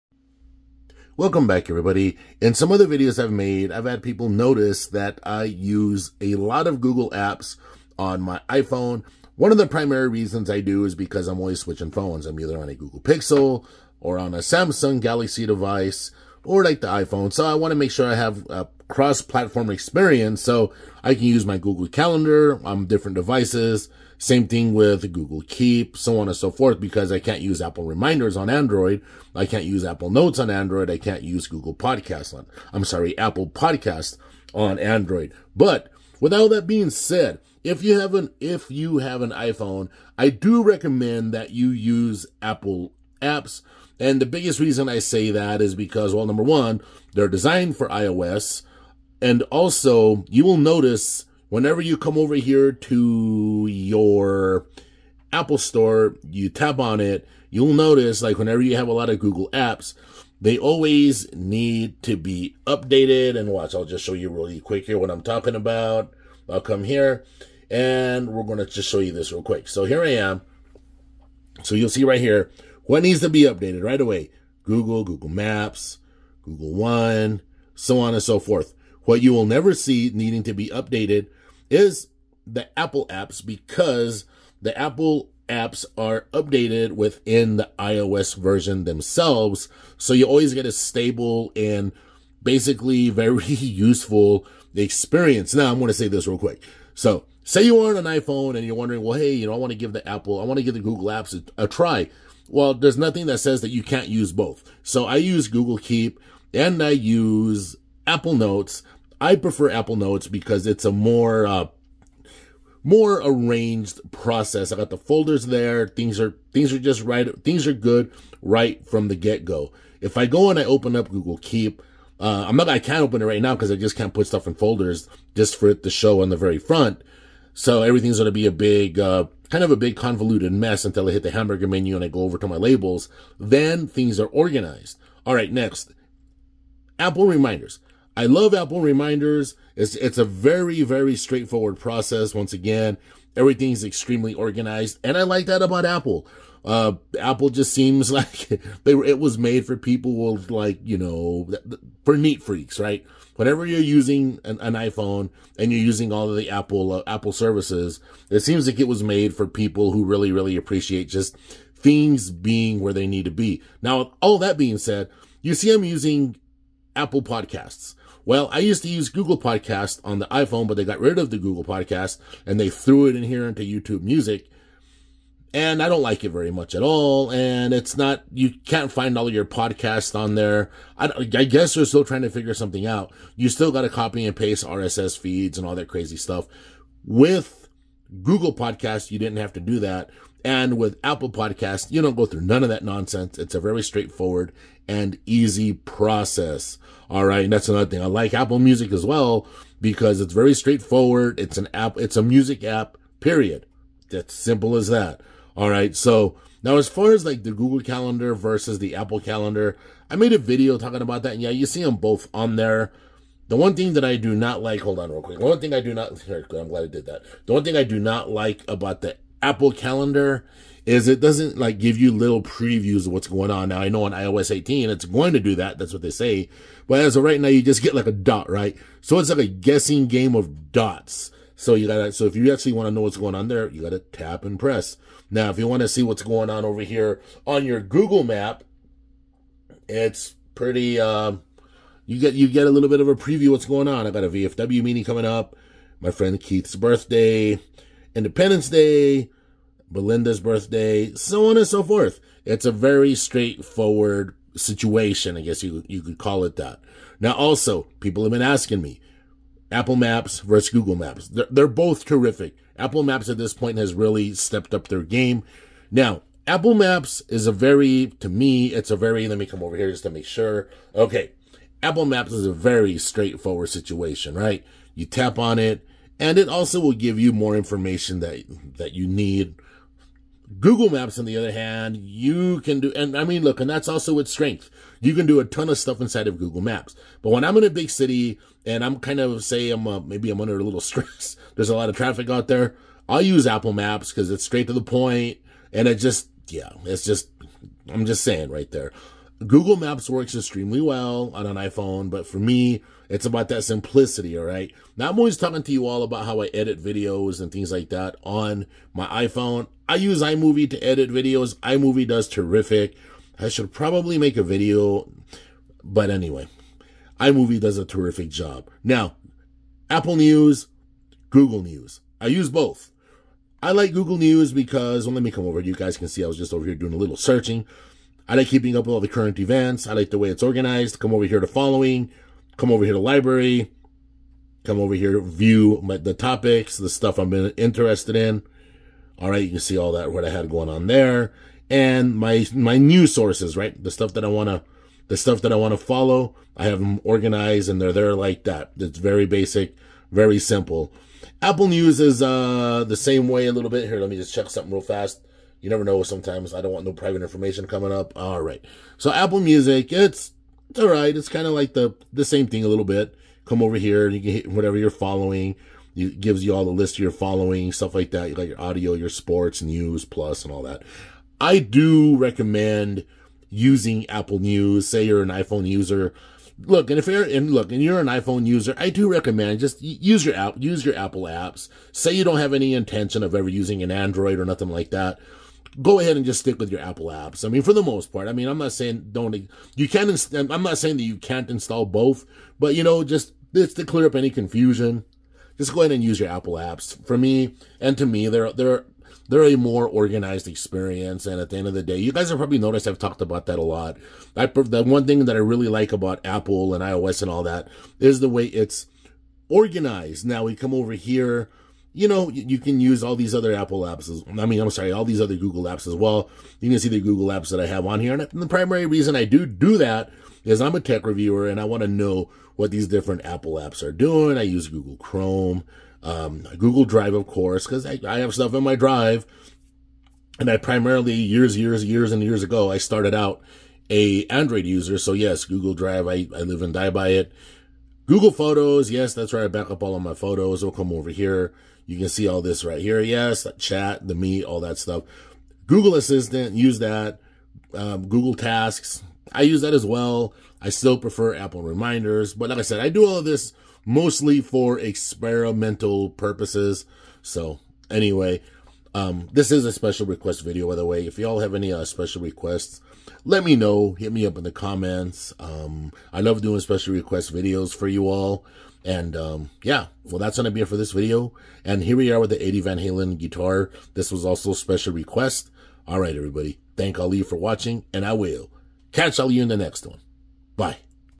Google Apps vs Apple Apps
In this video I'm going to give an explanation as to why I use certain Google Apps on my iPhone and why I use certain Apple apps on my iPhone.